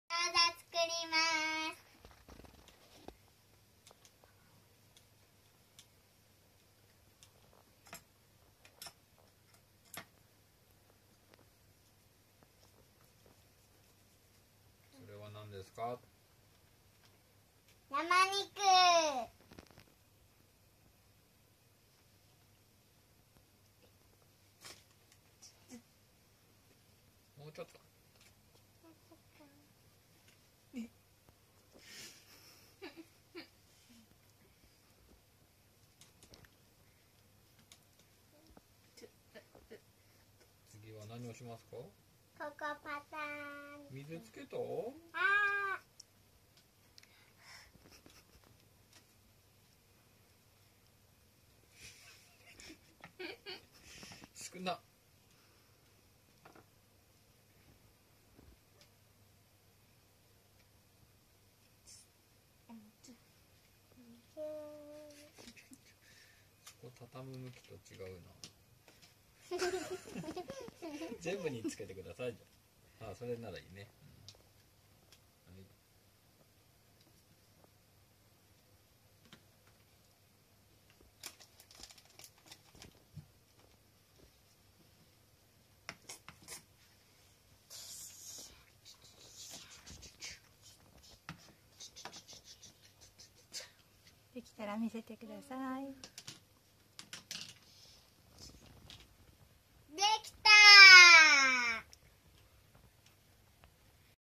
お兄ちゃんたちとギョウザ作り
おウチの台所でお兄ちゃん2人と楽しく夕食用のギョウザ作りをしました。1つ作業を忘れたことに気づいた時の長男（左）と志守葉のシンクロした動きにご注目です。